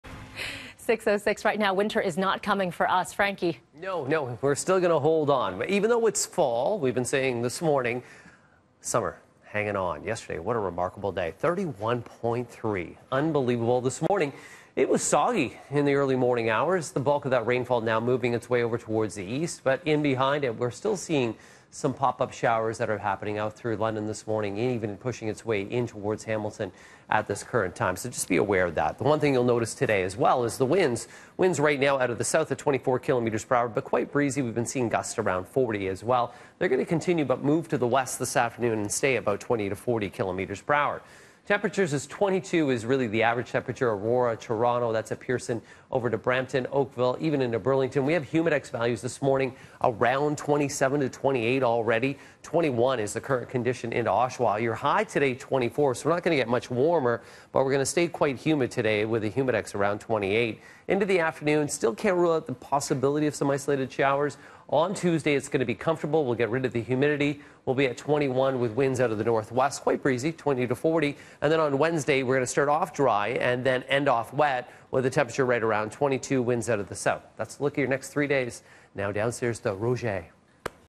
Summer hanging on for the first day of fall
Partly cloudy on Monday with a slight risk of showers in the afternoon, high 24 C with a humidex of 28. CityNews weather specialist Frank Ferragine has the forecast details.